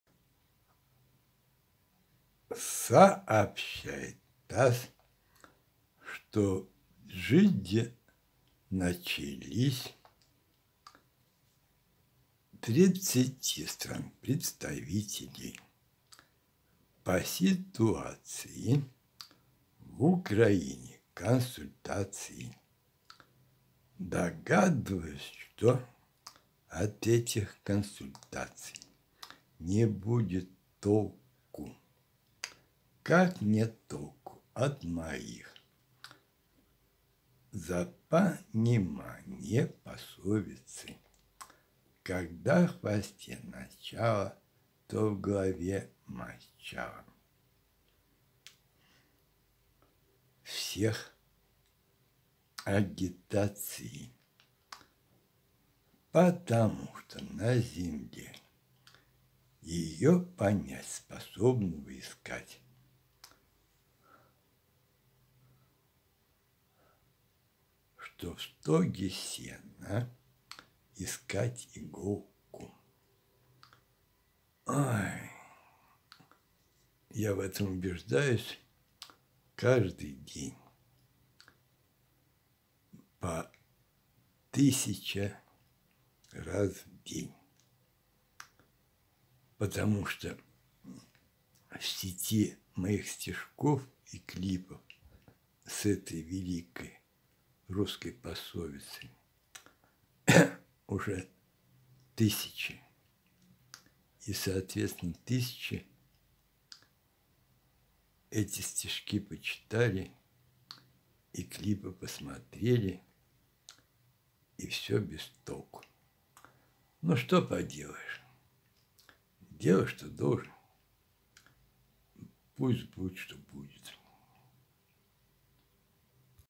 О в Джидде консультациях сообщает ТАСС
Сообщает ТАСС, что в Джидде начались тридцати стран представителей по ситуации в Украине консультации.
Догадываюсь, что от этих консультаций не будет толку.
Как нет толку от моей за понимание пословицы – «Когда в хвосте начало, то в голове мочало» – всех агитации.
Потому что на Земле ее понять способного искать, в стоге сена что искать иголку.